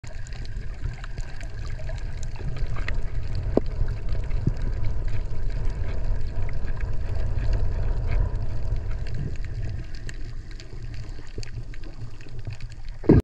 Eagle Ray from underwater.
An Eagle ray viewed from below and to it’s right in coral reef area.